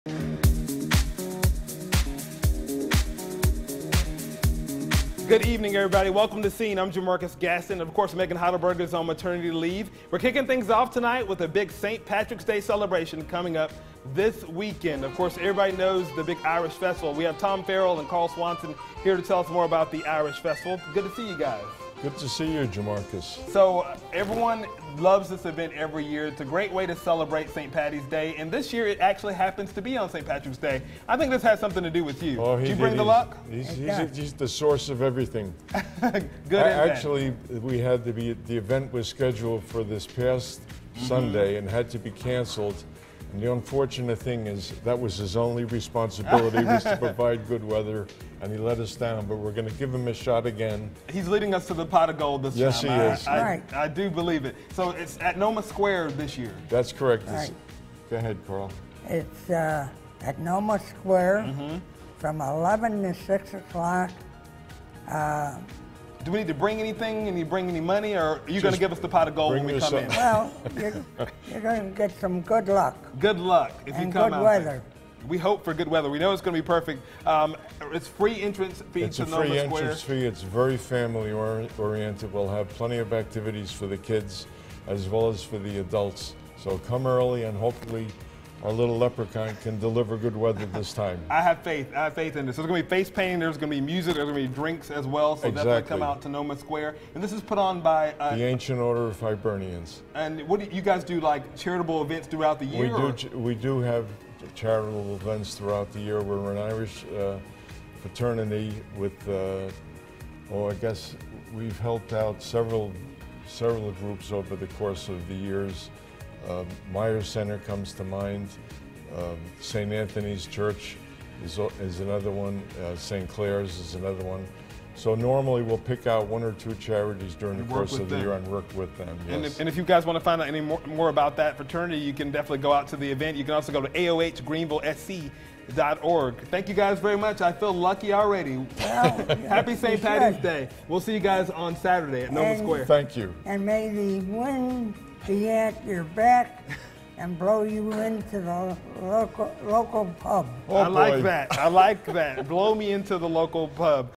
The Irish Festival
There is a big St. Patrick's Day celebration coming up this weekend! Tom Farrell and Carl Swanson are here to tell us about "The Irish Festival"! The festival will be held at NOMA Square, with Irish-American cuisine from chefs at the Hyatt Hotel, Irish music, Irish dancing, face-painting, balloon art, inflatable slides, and more.